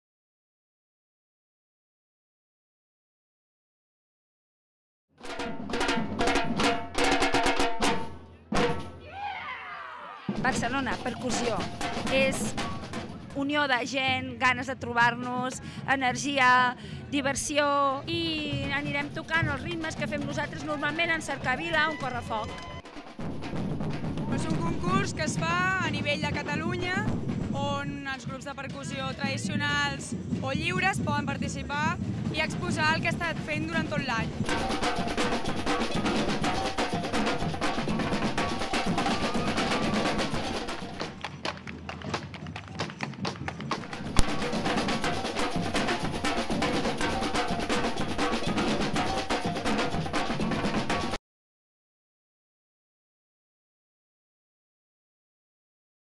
Al so del timbal
El Guinardó ha celebrat el 9è Concurs Barcelona Percussió, que ha aplegat percussionistes vinguts d'arreu de Catalunya.